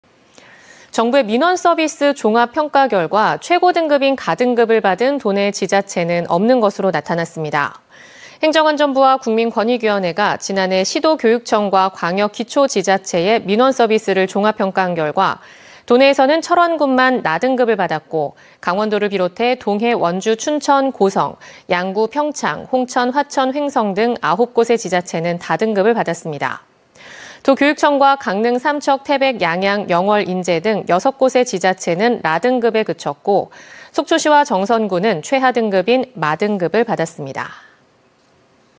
[단신] 강원자치도·교육청·지자체, 민원 평가 '최고 등급 한 곳도 없어' 20240219
정부의 민원서비스 종합평가 결과
최고 등급인 가 등급을 받은 지자체는
없는 것으로 나타났습니다.

행정안전부와 국민권익위원회가 지난해
시도교육청과 광역·기초 지자체의
민원 서비스를 종합 평가한 결과
도내에서는 철원군만 나 등급을 받았고
강원도를 비롯해 동해·원주·춘천·고성·
양구·평창·홍천·화천·횡성 등 9곳의 지자체는
다 등급을 받았습니다.

도교육청과 강릉·삼척·태백·양양·영월·인제 등
6곳의 지자체는 라 등급에 그쳤고
속초시와 정선군은 최하 등급인 마 등급을 받았습니다.